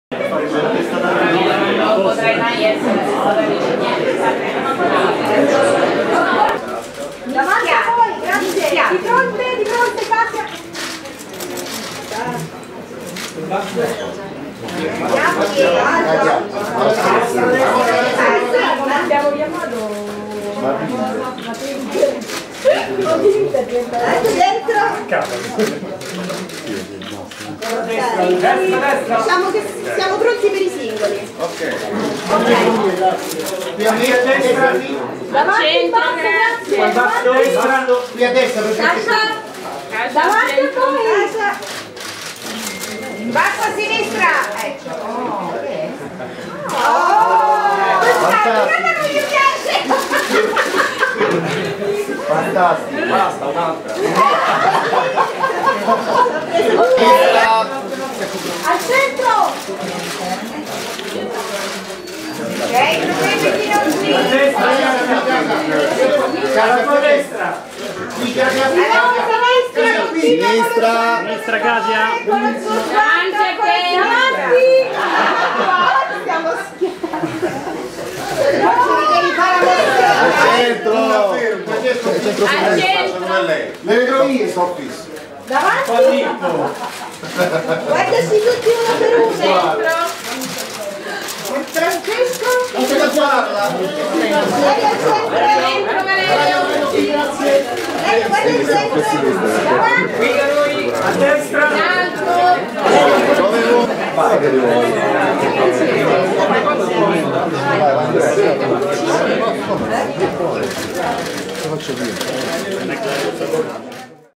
moglie e marito - il photocall
Pierfrancesco Favino e Kasia Smutniak sono i protagonisti del film Moglie Marito, al cinema dal 12 aprile 2017